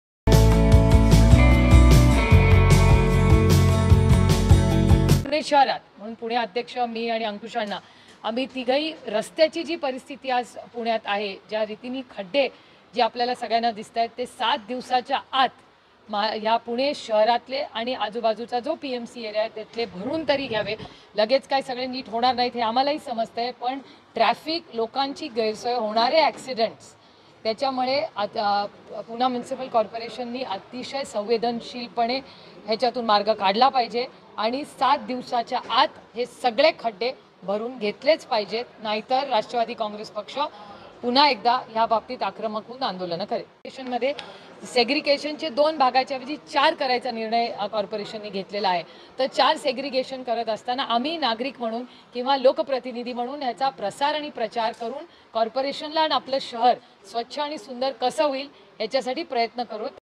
"Potholes in Pune should be filled within seven days!" — Supriya Sule, MP, NCP
Pune: Today, Supriya Sule, MP, NCP during her Pune visit reviewed the status of roads in the city. To say Sule was displeased would be an understatement of the millennium. Supriya also checked the garbage problem at Bavdhan and reviewed the bridge work on Sinhgad road. Picking  up the cudgel, she caught up with Pune Municipal Commissioner Vikram Kumar for a discussion. At the meeting, Sule demanded that the potholes on the roads should be filed within seven days within the PMC precincts.